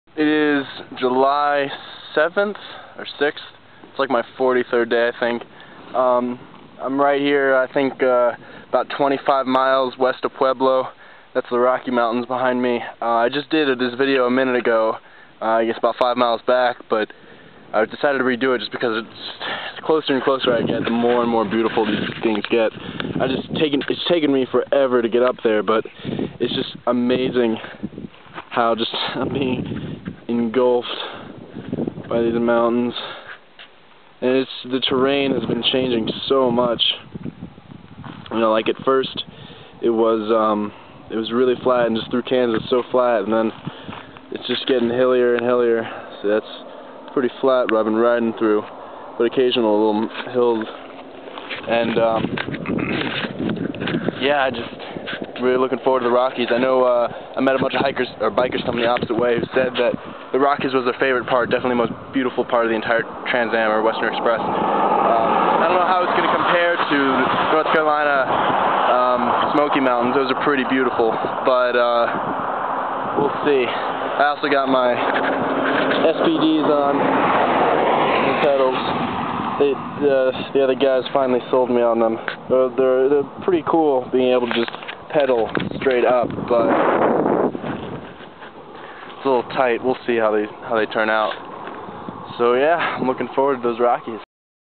Day #43
I took this video during my cross-country bicycle tour from Savannah, GA to Berkeley, CA. I'm a few dozen miles west of Pueblo, CO--just at the base of the Rockey Mountains.